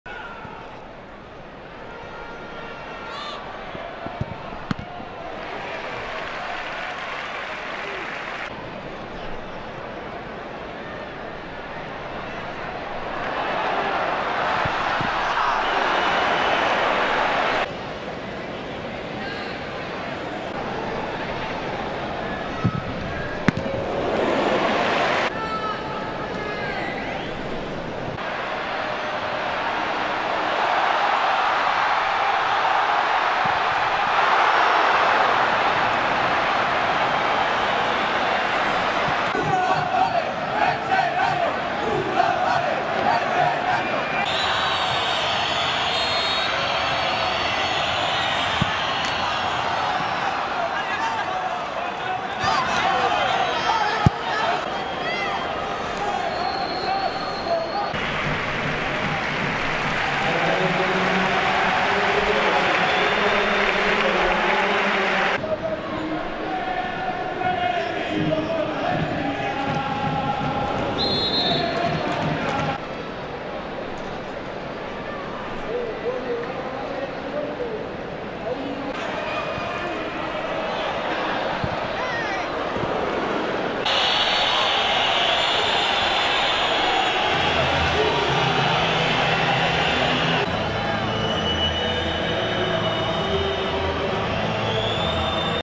Resumen de Málaga CF vs Getafe CF (0-1)
Loïc Rémy firma la victoria del Getafe CF en La Rosaleda en el partido que cerraba la temporada LaLiga Santander J38